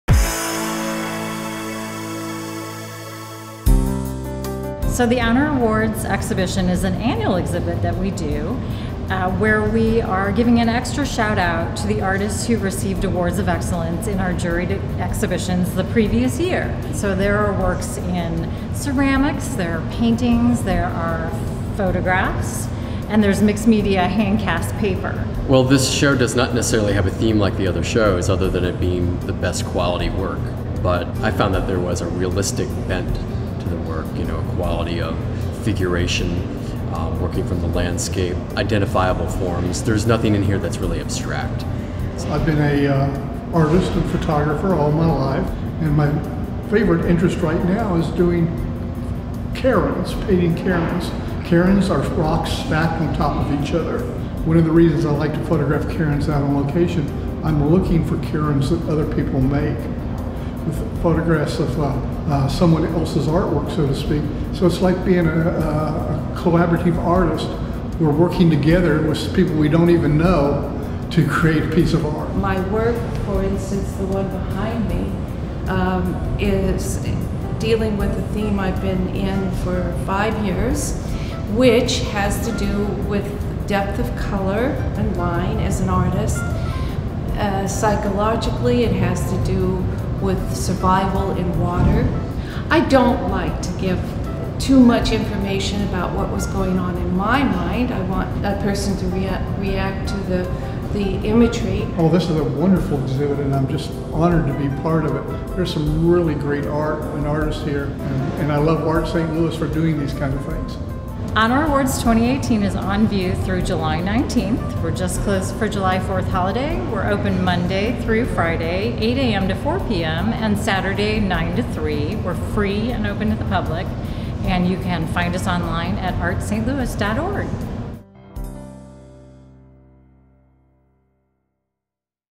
Top 10 Regional Artists Selected for Art Saint Louis Honor Awards Exhibit
Honor Awards 2018 at Art Saint Louis features recent artworks by ten St. Louis regional artists from Missouri and Illinois. The 48 artworks in this exhibition include ceramic sculpture, handcast paper, mixed media, painting, and photography.